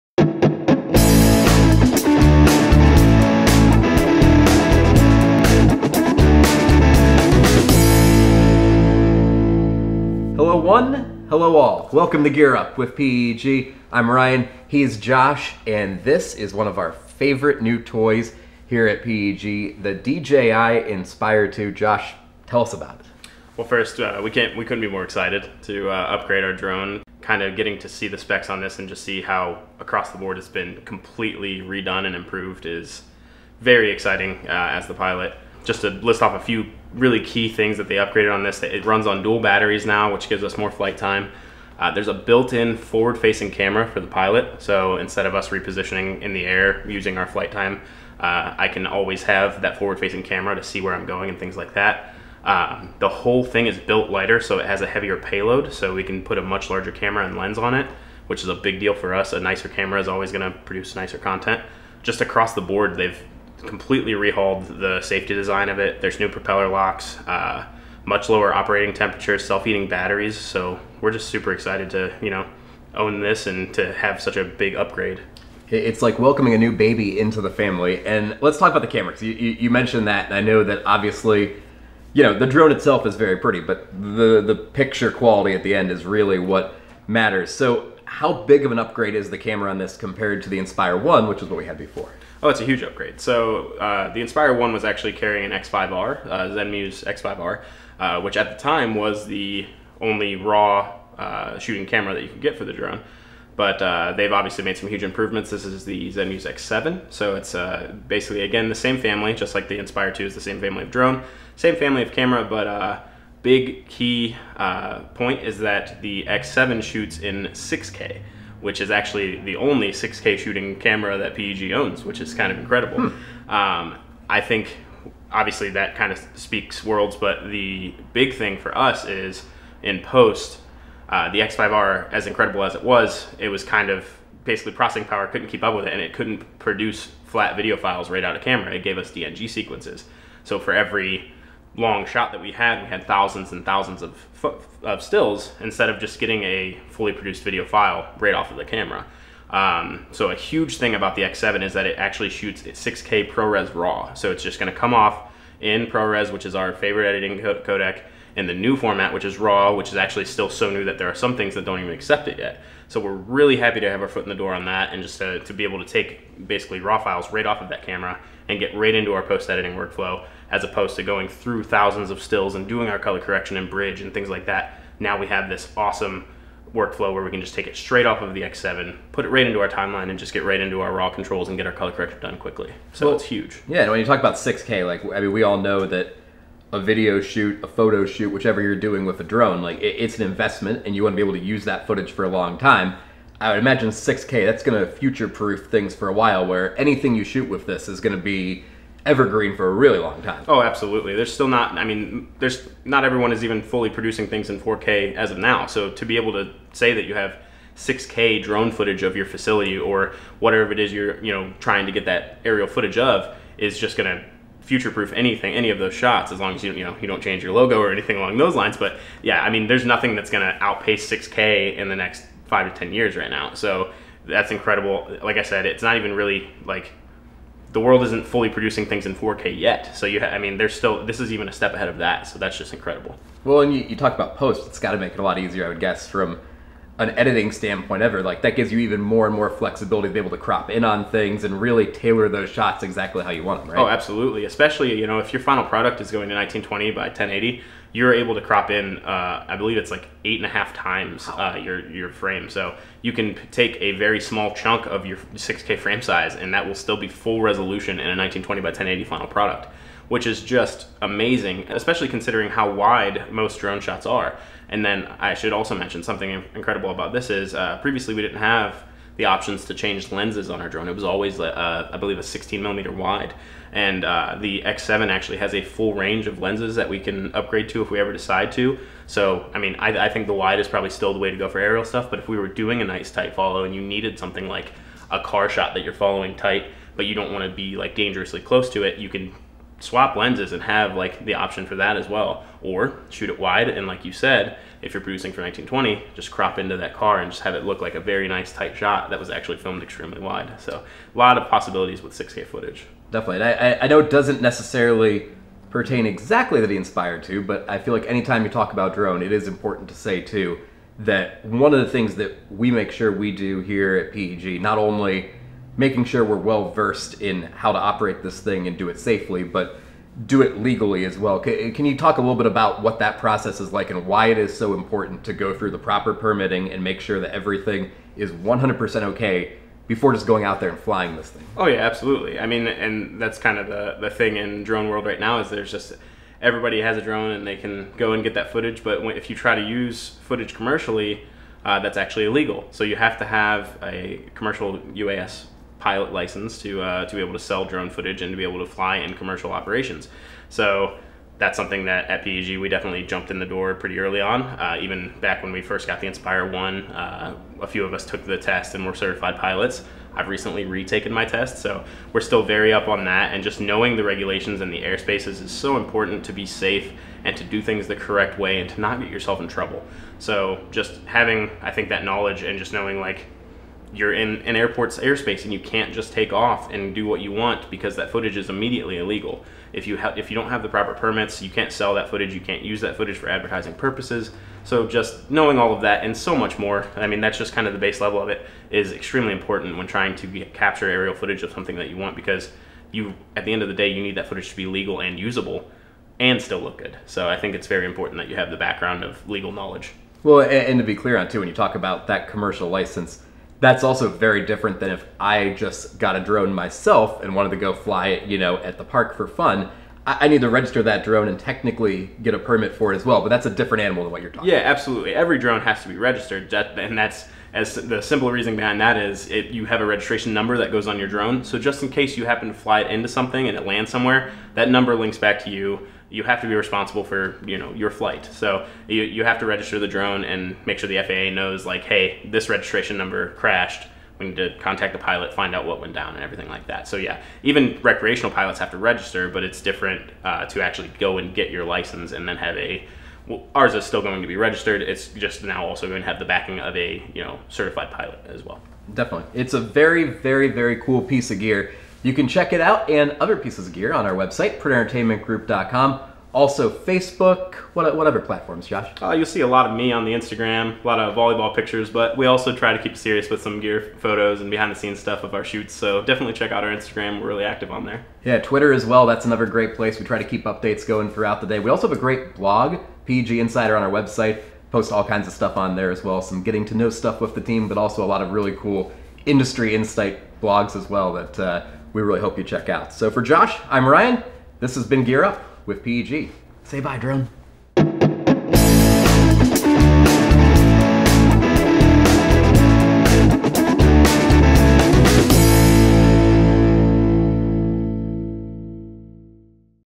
Gear Up with PEG: DJI Inspire 2
From longer flight times to 6K shooting abilities, Josh talks to Ryan about everything we love about our DJI Inspire 2.